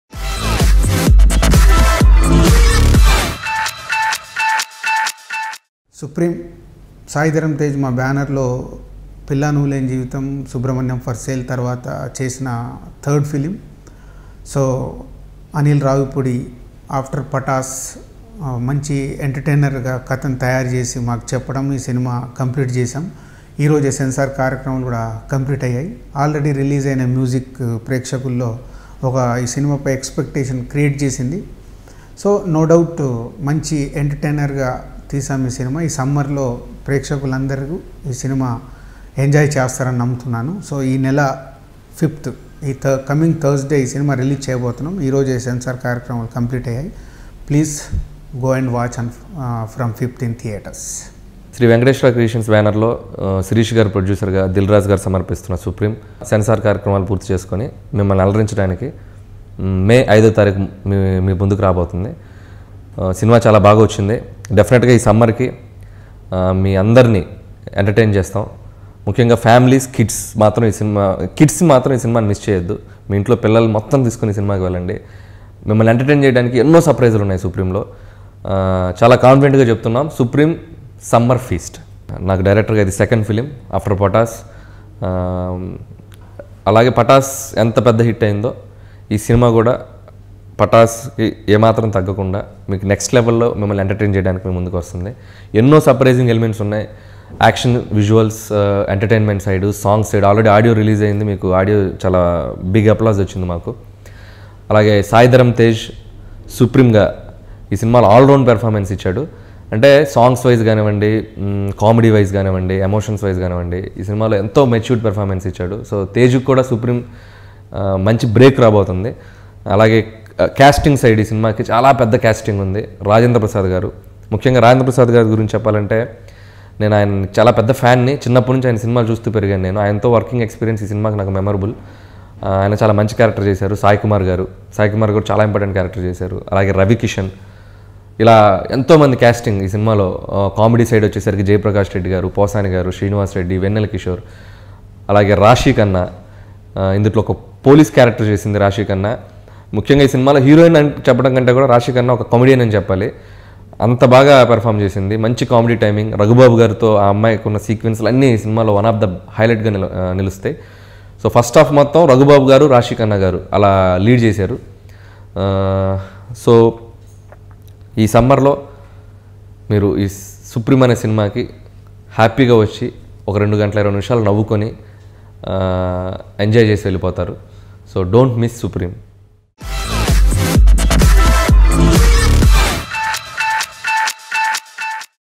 Dil Raju & Anil Ravipudi About Supreme Movie Release | Sai Dharam Tej | Rashi khanna
Watch Dil Raju & Anil Ravipudi About Supreme Movie Release
directed by Anil Ravipudi.
produced by Dil Raju. music by Sai Prakash.
starring: Sai Dharam Tej & RAshi Khanna